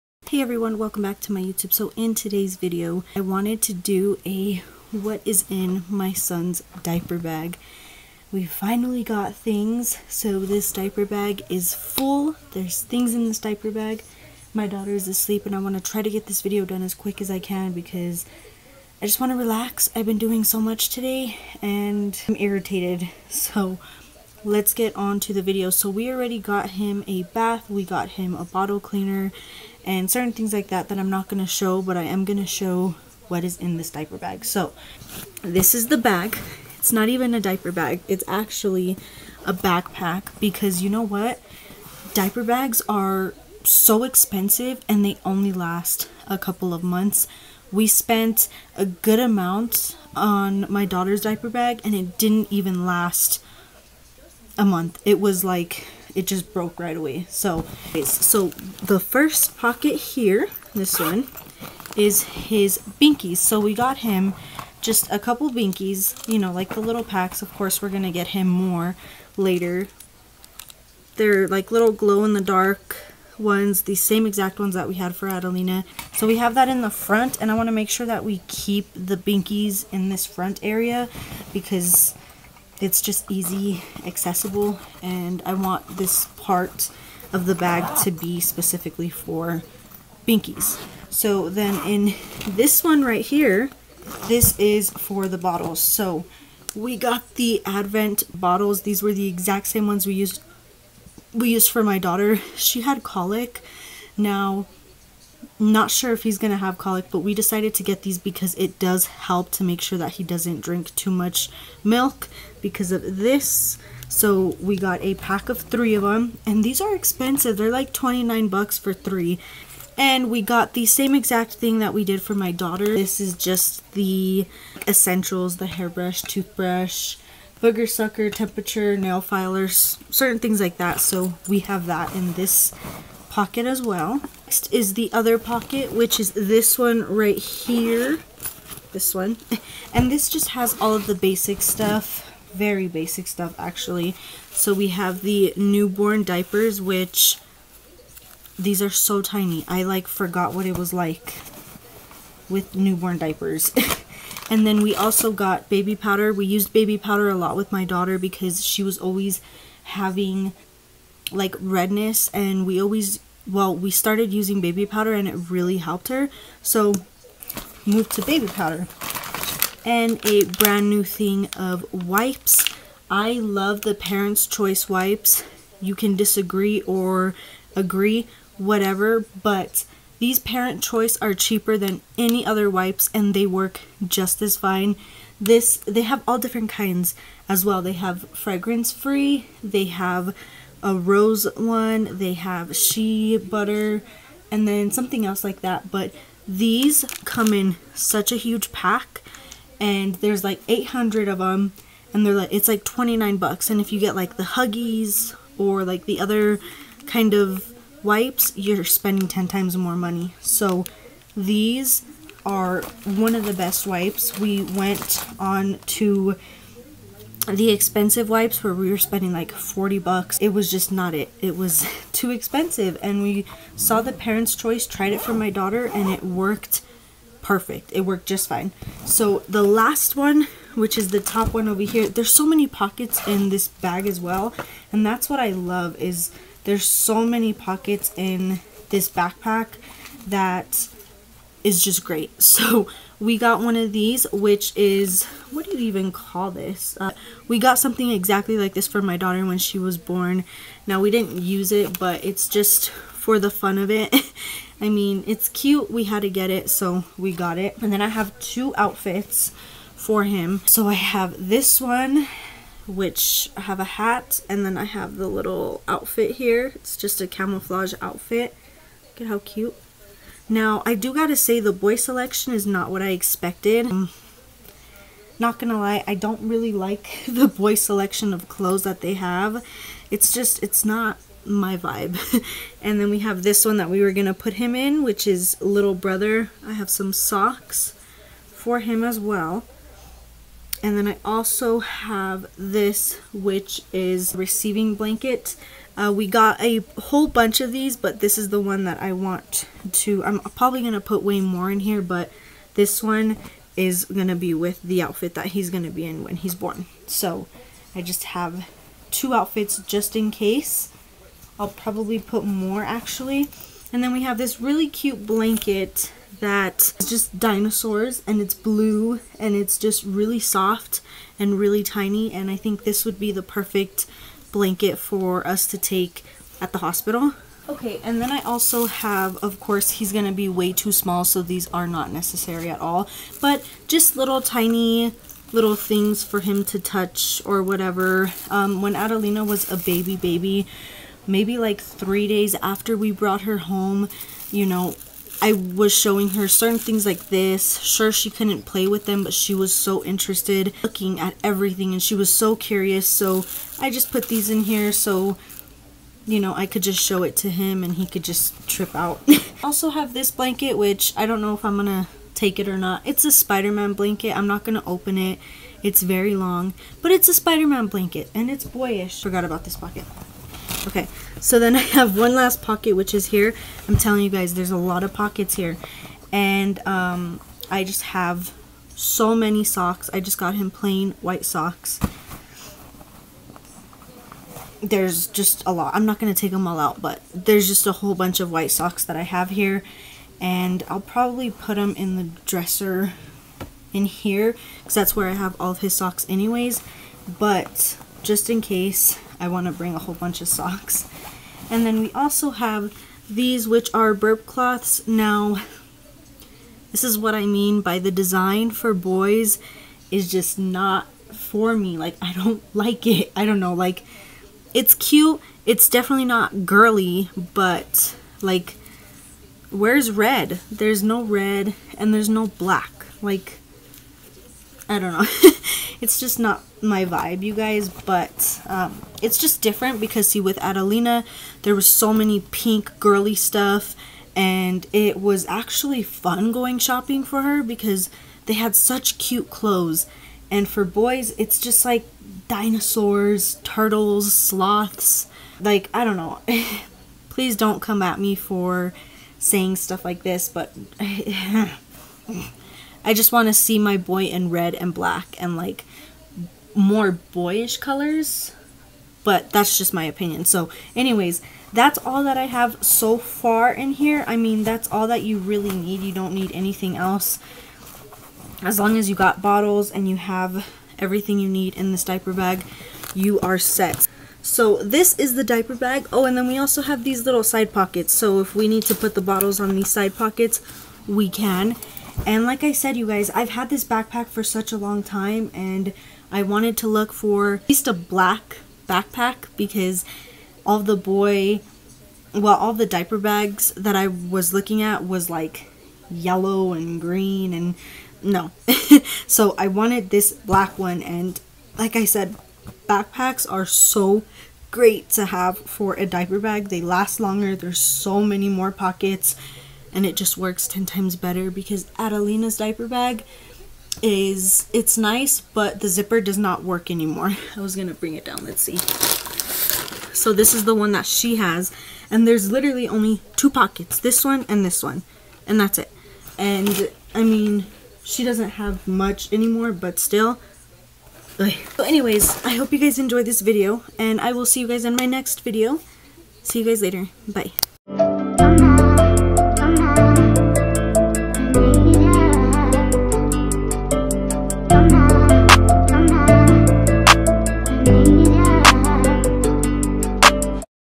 What’s in my Diaper Bag/Hospital Bag
Please keep in mind I will NOT be breastfeeding so this is all the things I need in my diaper bag. If you are breastfeeding of course you will need more. Also please excuse the over editing I was in slightly a rush when trying to upload this video 🤷‍♀️ it happens